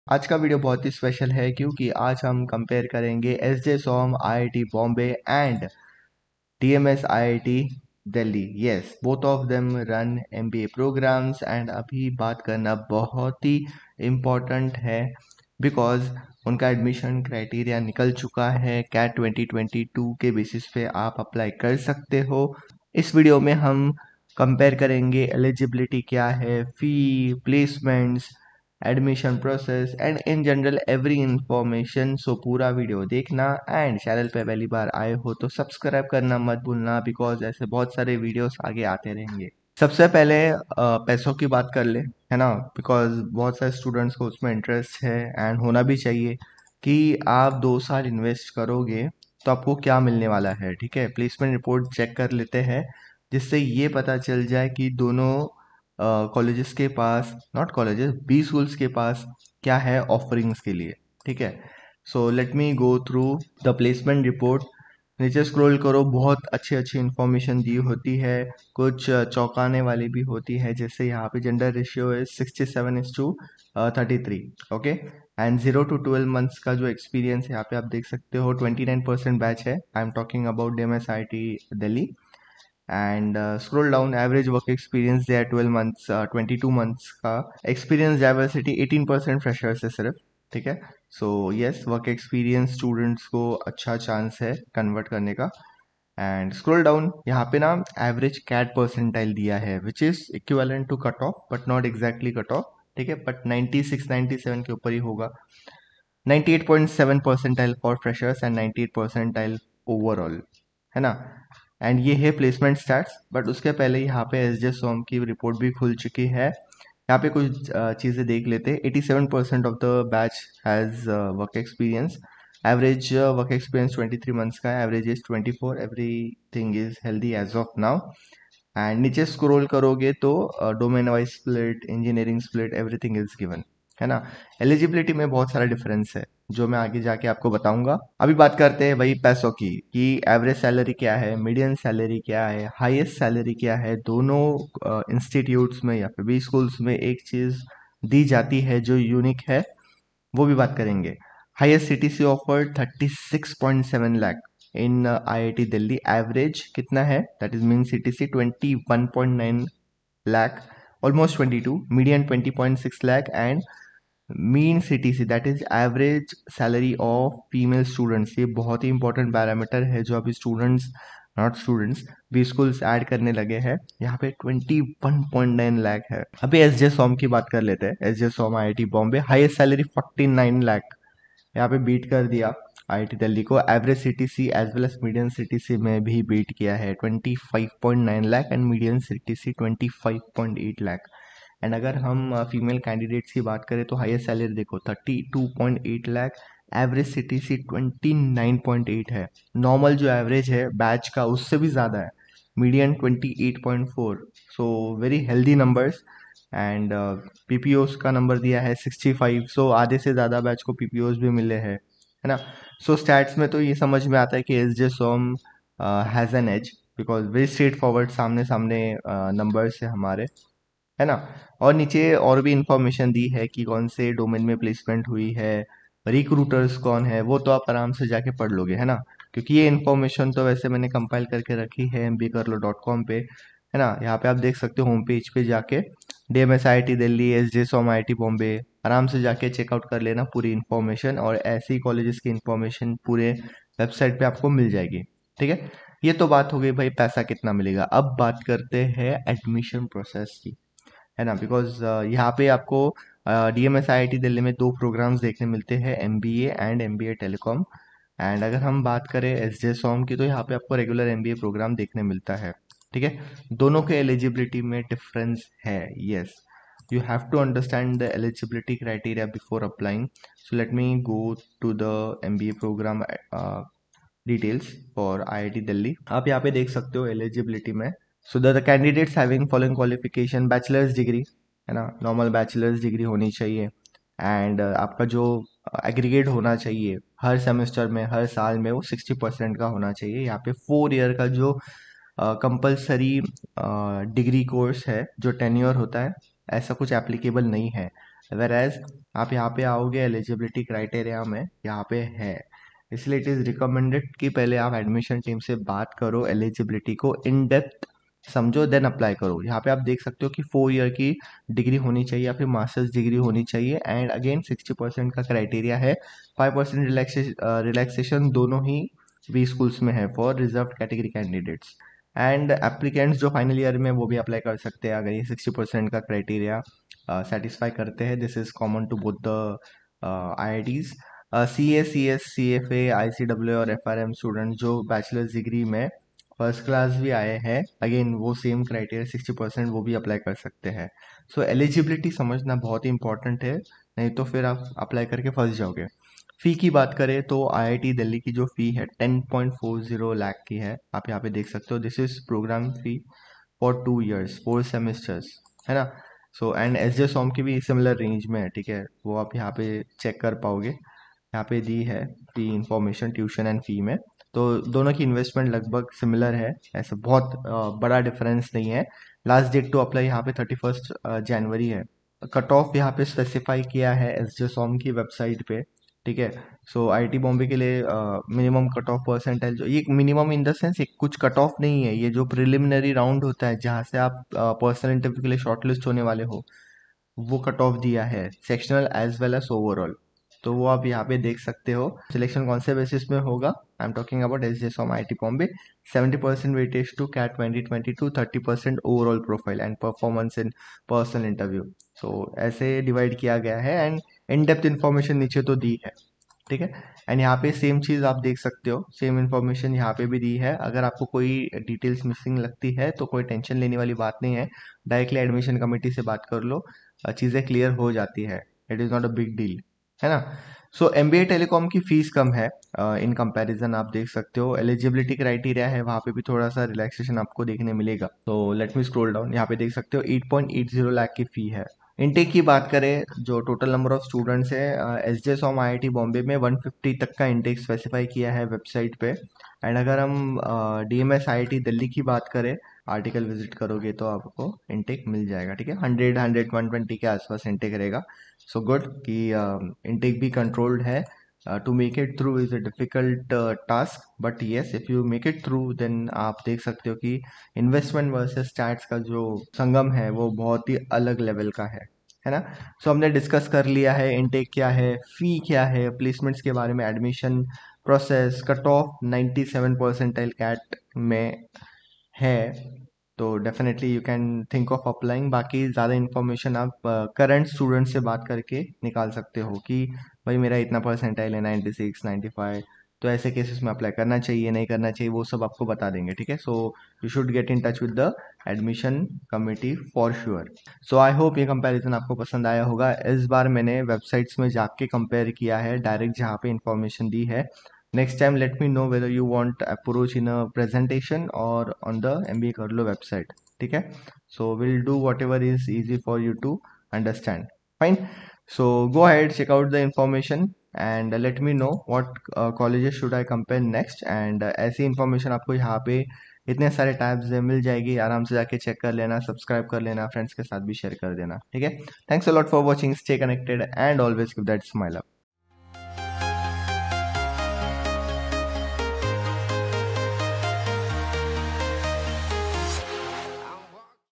IIT Bombay Vs IIT Delhi - Which is better for MBA | SJMSOM IIT Bombay Vs DMS IIT Delhi | MBA 2023
IIT Bombay Vs IIT Delhi  - Which is better for MBA | SJMSOM IIT Bombay Vs DMS IIT Delhi | MBA from IITs